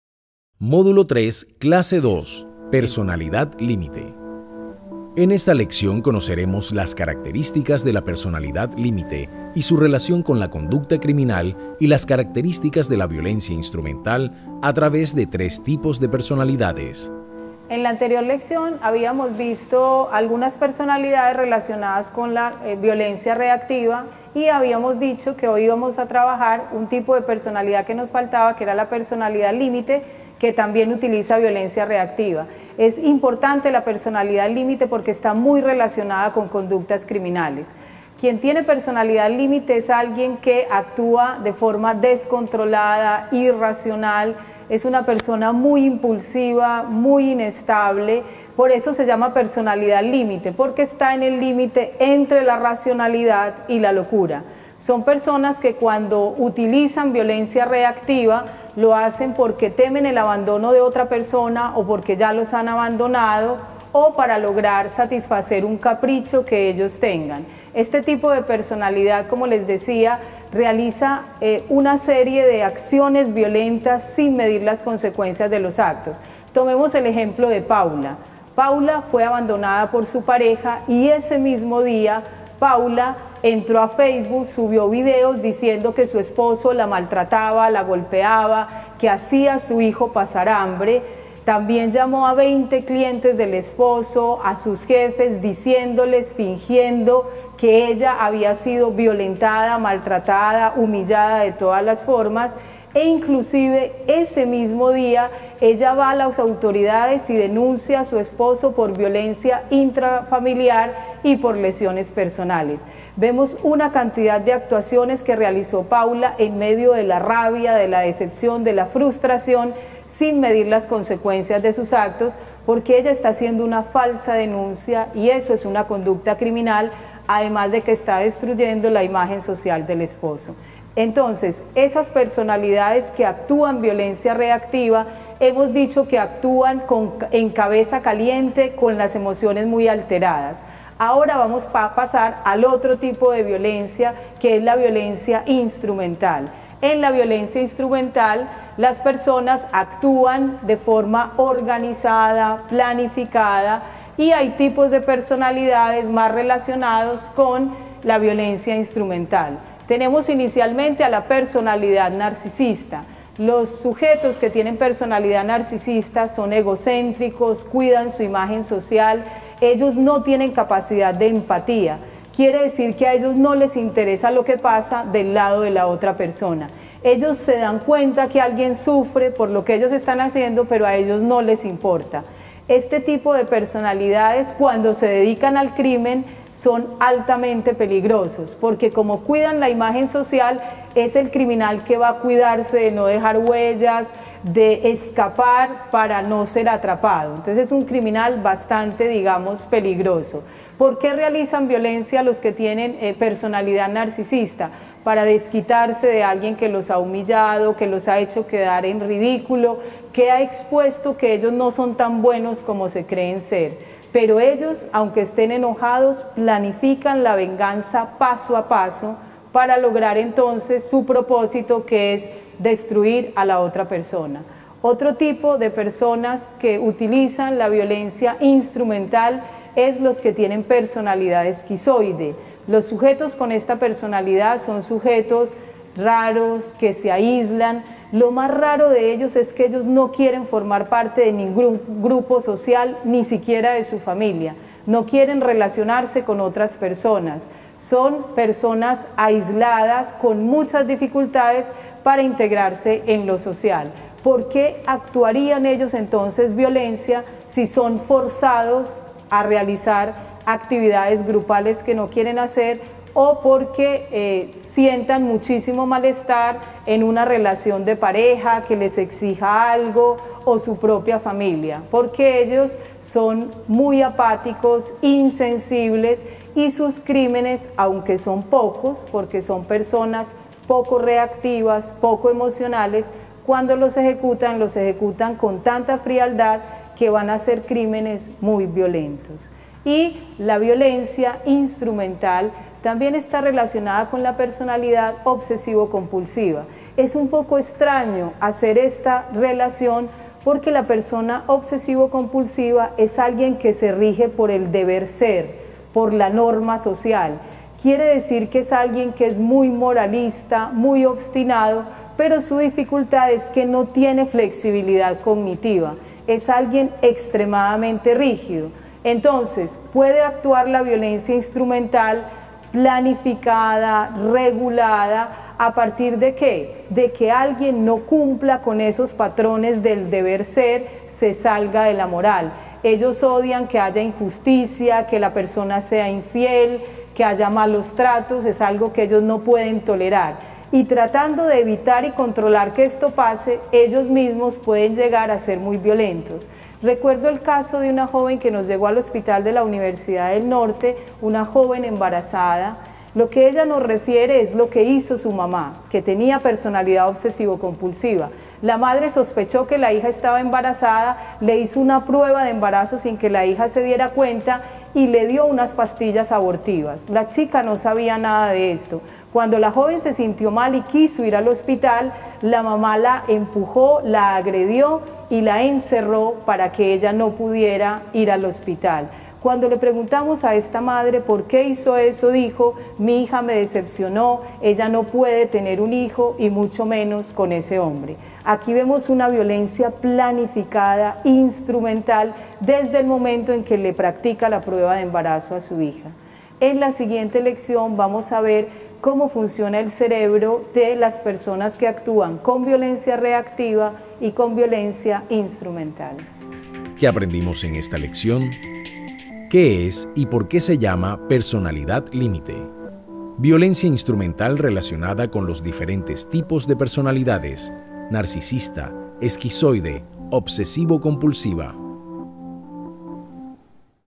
Psicopatía criminal - Módulo 3, lección 2
Lección 2, módulo 3, del curso gratuito  "Introducción a la psicopatía criminal" ,  MOOC de la Universidad del Norte.